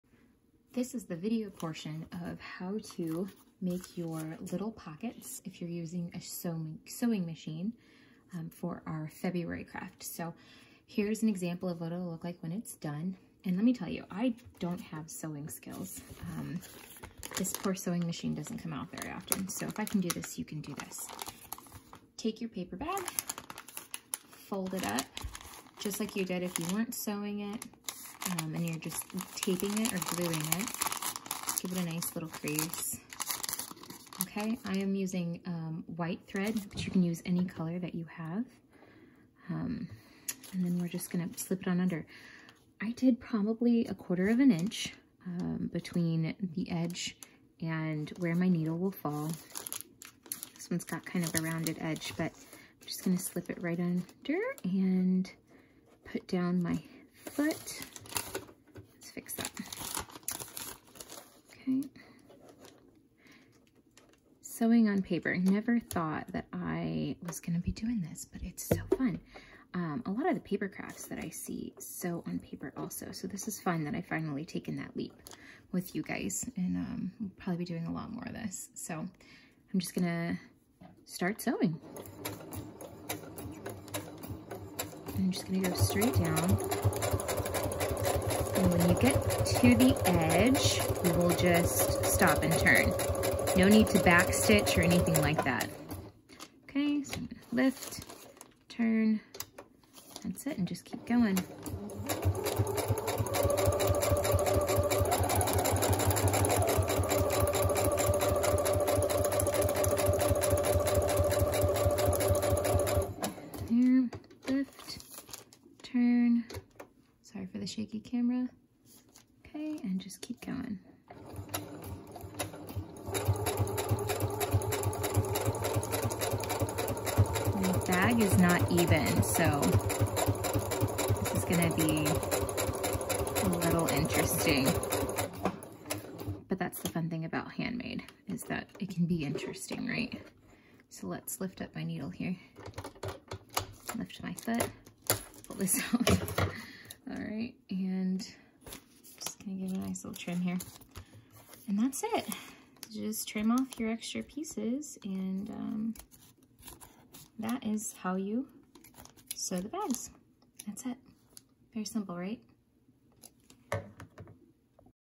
WPSC February Craft: Sewing the bag
Break out the sewing machine for a quick, easy demonstration of sewing on paper.